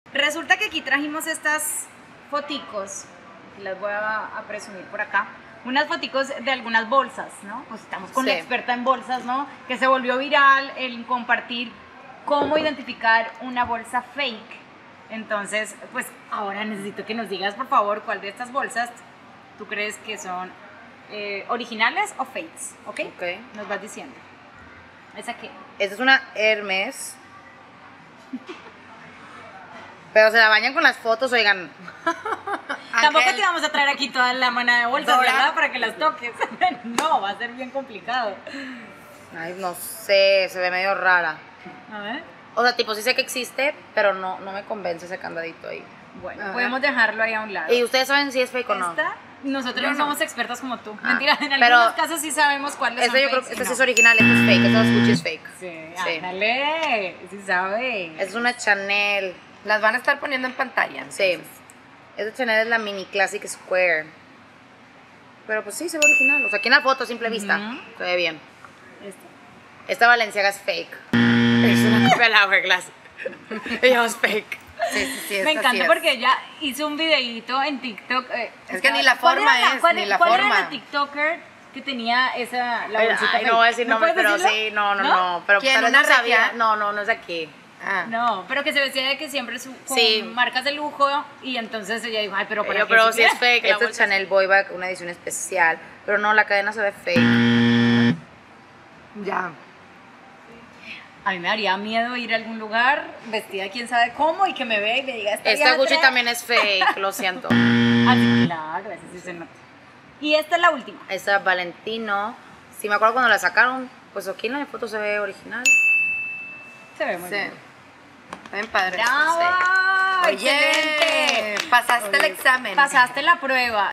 ¿Real o Fake? El reto de las bolsas de lujo con Priscy Escoto
Jugamos el reto de las bolsas con Priscy Escoto para poner a prueba su conocimiento de marcas de lujo